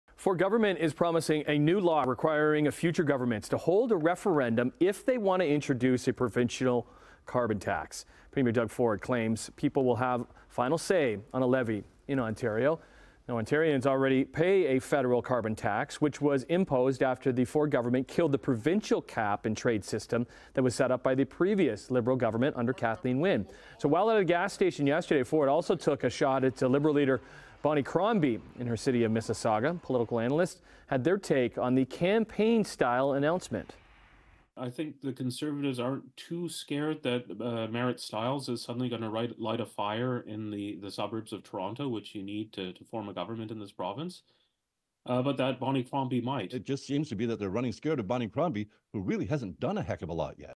Ford uses announcement of carbon tax referendum bill to take digs at Bonnie Crombie
The Ford government is promising a new law requiring future governments to hold a referendum if they wish to introduce a provincial carbon tax.